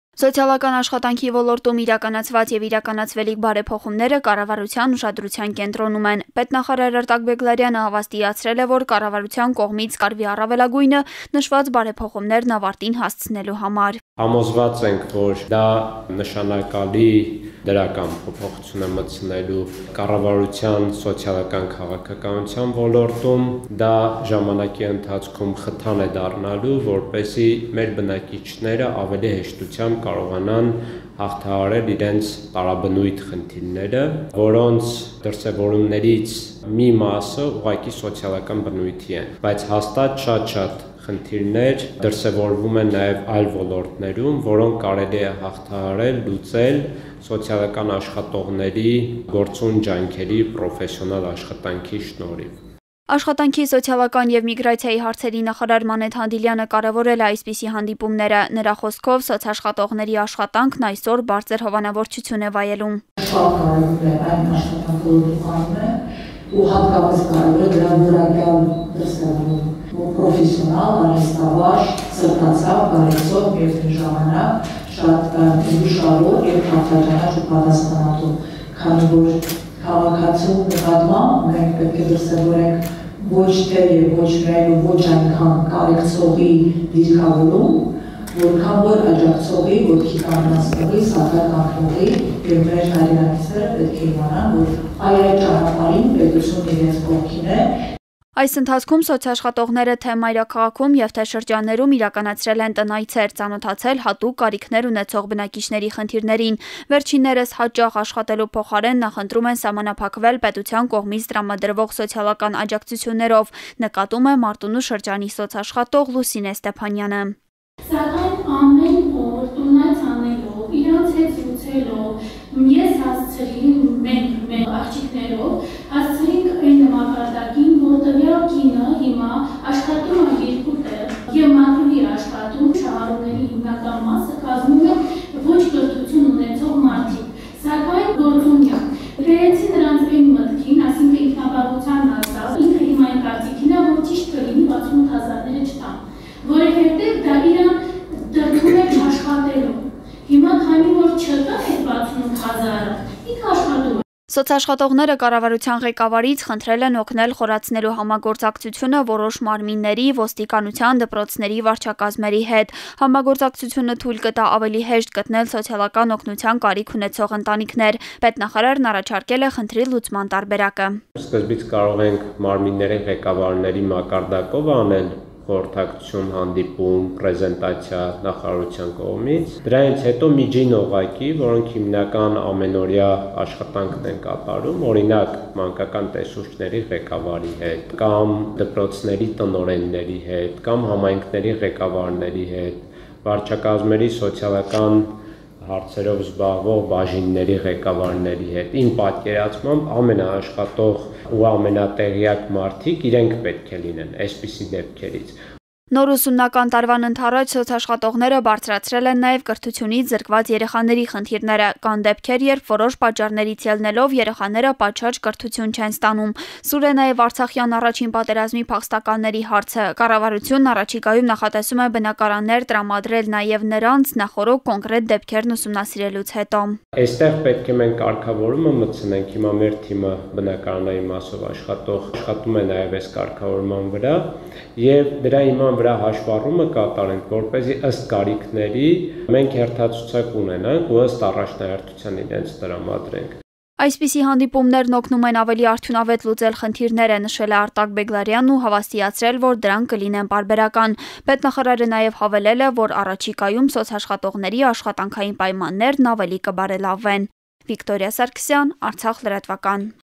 📻📌Սոցիալական ոլորտի բարեփոխումները կառավարության առաջնային նպատակներից է. 20.08.2021
Պետական նախարար Արտակ Բեգլարյանի հանդիպումն այսօր սոցիալան աշխատողների հետ էր։ Թեման՝ սոցիալական աշխատանքի ոլորտում ինստիտուցիոնալ բարեփոխումների առաջին փուլի արդյունքերը։ Քննարկվել են այս ժամանակաշրջանում իրականացված աշխատանքները։ Սոցիալական աշխատողները բարձրաձայնել են իրենց խնդիրներն ու առաջարկները։
📻👇Մանրամասները՝ Վիկտորյա Սարգսյանի ռեպորտաժում: